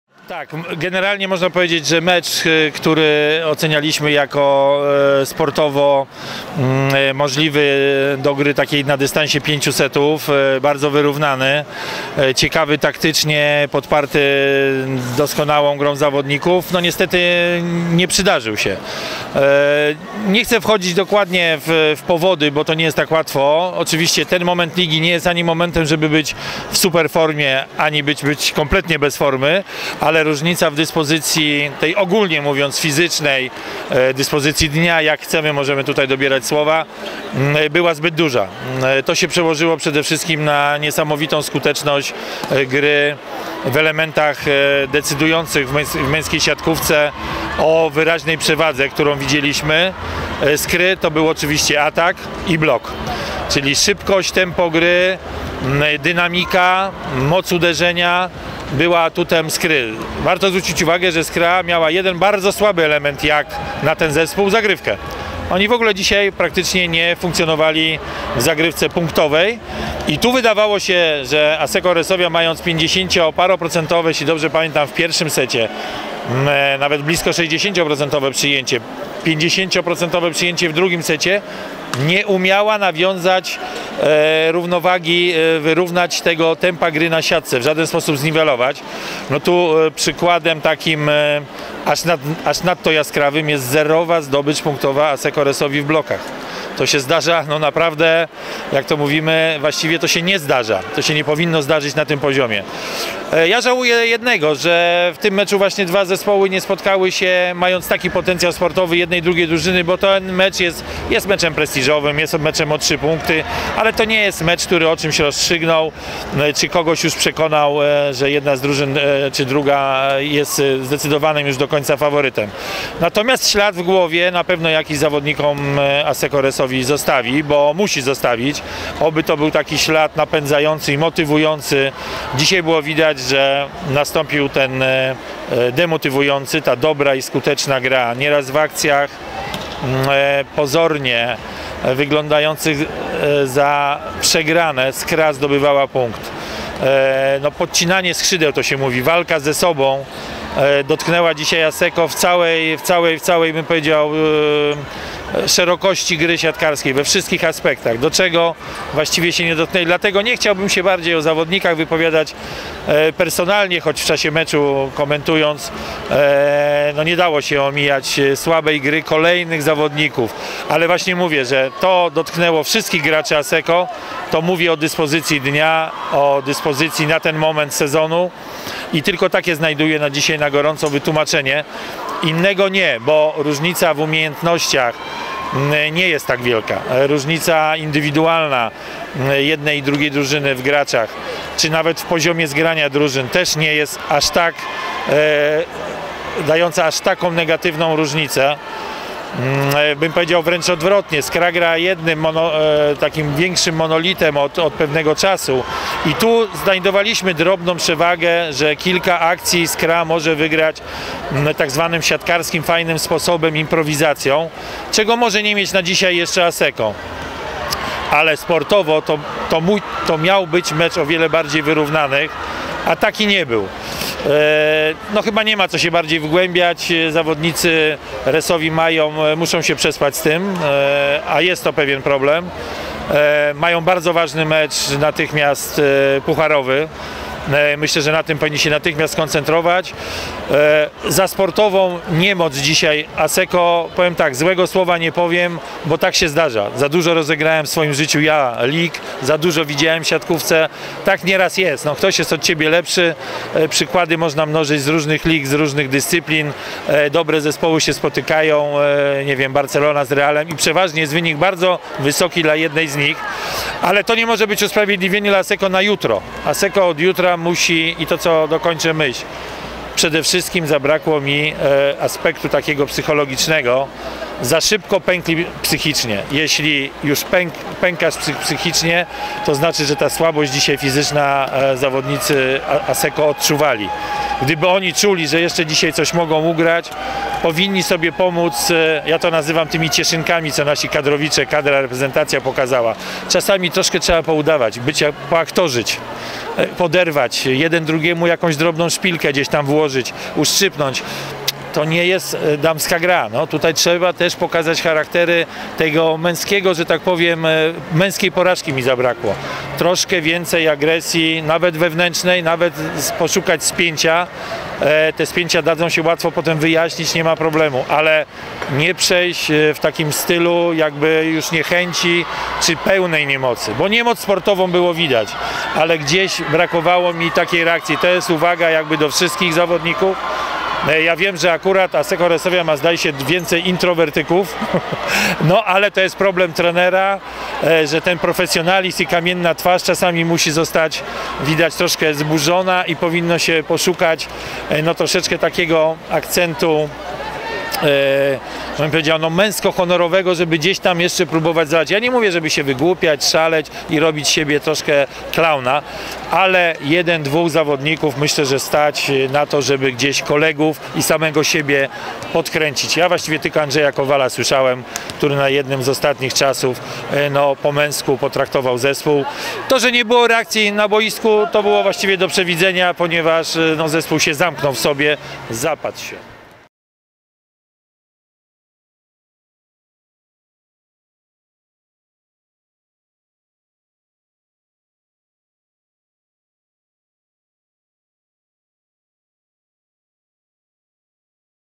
PGE Skra Bełchatów vs Asseco Resovia Rzeszów w opinii Wojciecha Drzyzgi
Komentator Polsatu Sport, a prywatnie tato naszego rozgrywającego - Wojciech Drzyzga podzielił się z nami swoimi obiektywnymi spostrzeżeniami o pojedynku w łódzkiej Atlas Arenie.